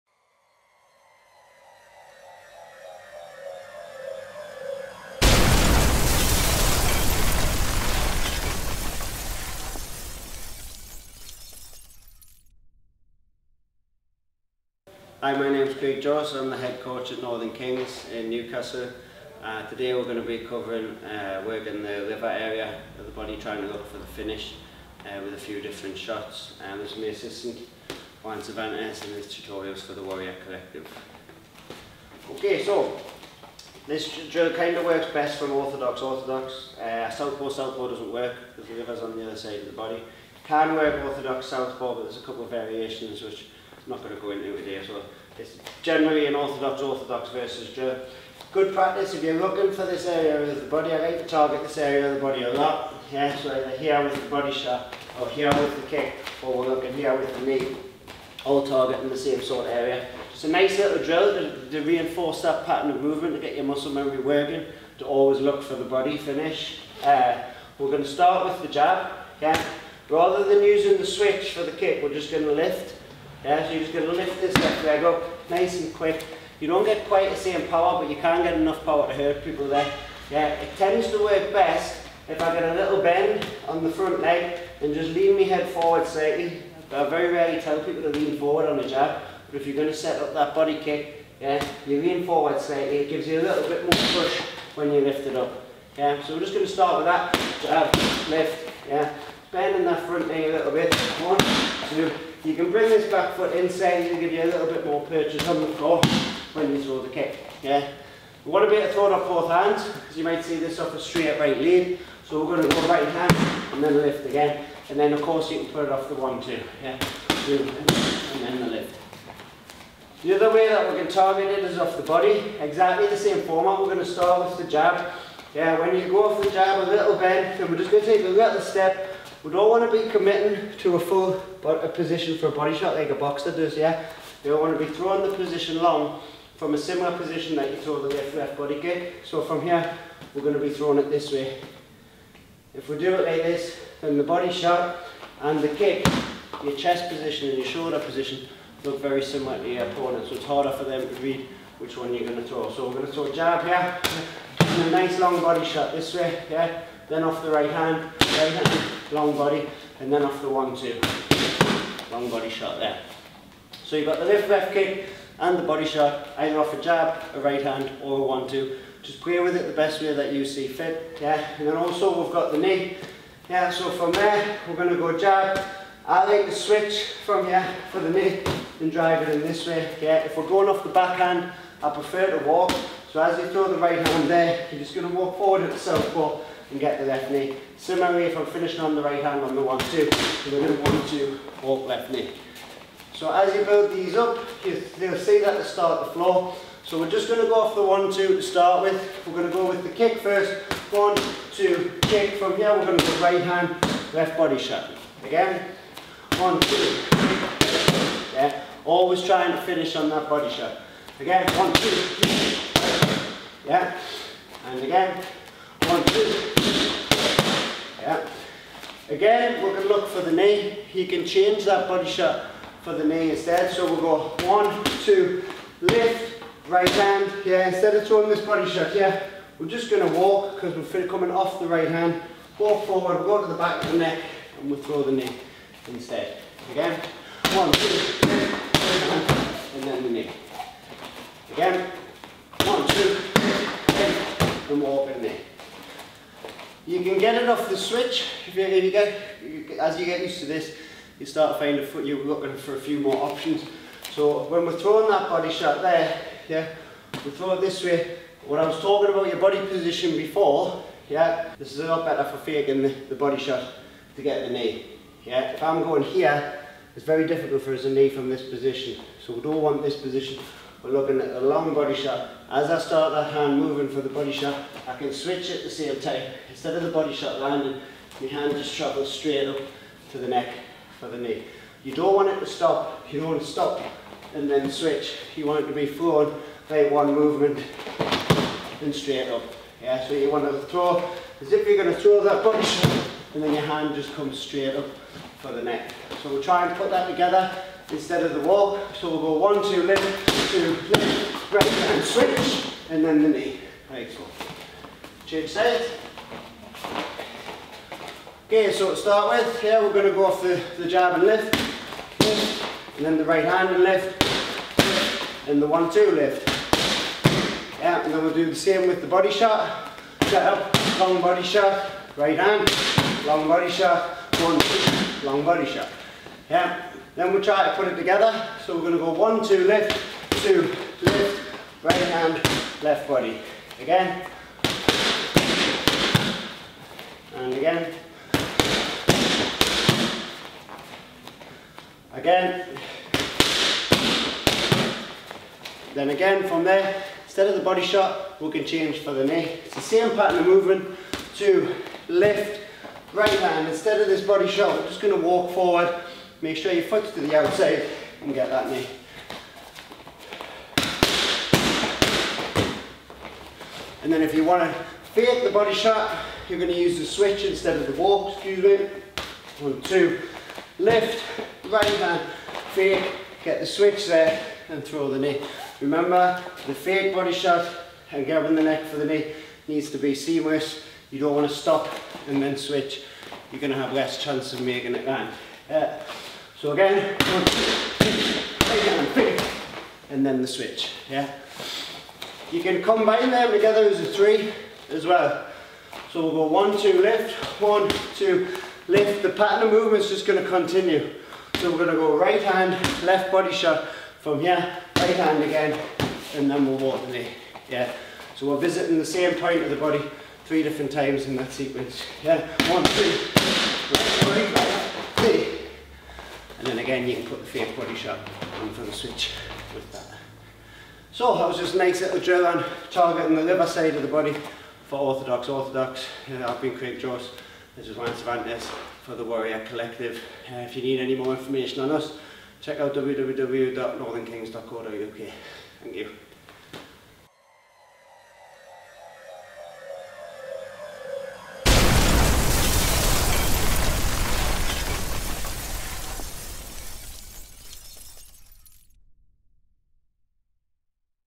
Muay Thai Liver Destruction Framework Tutorial
Craig Jose, head instructor of Northern Kings in Newcastle is here filmed by the Warrior Collective giving a tutorial on a framework to systematically target/damage the liver he likes to coach to his students and fighters in Muay Thai.